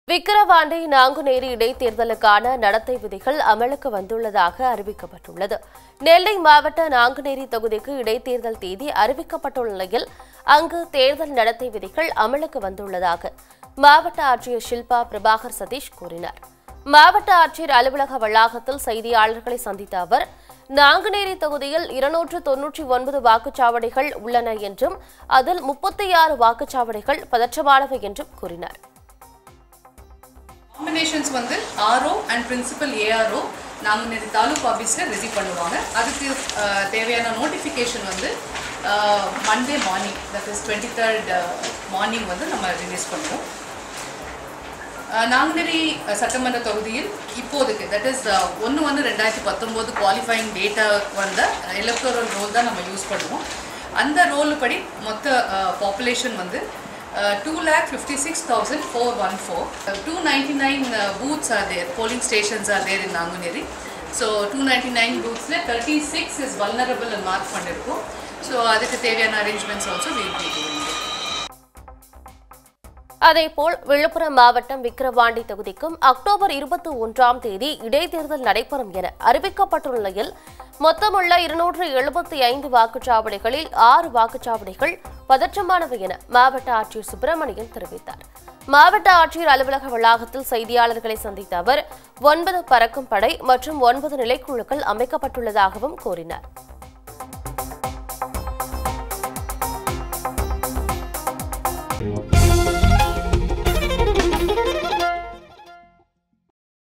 விக்கிரவாண்டி, நாங்குநேரி இடைத்தேர்தலுக்கான நடத்தை விதிகள் அறிவிப்பு!
விக்கிரவாண்டி, நாங்குநேரி இடைத்தேர்தலுக்கான நடத்தை விதிகள் அமலுக்கு  வந்துள்ளதாக அறிவிக்கப்பட்டுள்ளது.நெல்லை மாவட்ட நாங்குநேரி தொகுதிக்கு இடைத்தேர்தல் தேதி அறிவிக்கப்பட்டுள்ள நிலையில் அங்கு தேர்தல் நடத்தை விதிகள் அமலுக்கு வந்துள்ளதாக மாவட்ட ஆட்சியர் ஷில்பா பிரபாகர் சதீஷ் கூறினார்.மாவட்ட ஆட்சியர் அலுவலக வளாகத்தில் செய்தியாளர்களை சந்தித்த அவர், நாங்குநேரி தொகுதியில் 299 வாக்குச்சாவடிகள் உள்ளன என்றும் அதில் 36 வாக்குச்சாவடிகள் பதற்றமானவை என்றும் கூறினார். அதேபோல் விழுப்புரம் மாவட்டம் விக்கிரவாண்டி தொகுதிக்கும் அக்டோபர் 21ஆம் தேதி இடைத்தேர்தல்  நடைபெறும் என அறிவிக்கப்பட்டுள்ள நிலையில், மொத்தமுள்ள 275 வாக்குச்சாவடிகளில் 6 வாக்குச்சாவடிகள் பதற்றமனவை என மாவட்ட ஆட்சியர் சுப்பிரமணியன் தெரிவித்தார். மாவட்ட ஆட்சியர் அலுவலக வளாகத்தில் செய்தியாளர்களை சந்தித்த அவர், ஒன்பது பறக்கும்படை மற்றும் 9 நிலைக்குழுக்கள் அமைக்கப்பட்டுள்ளதாகக் கூறினார்.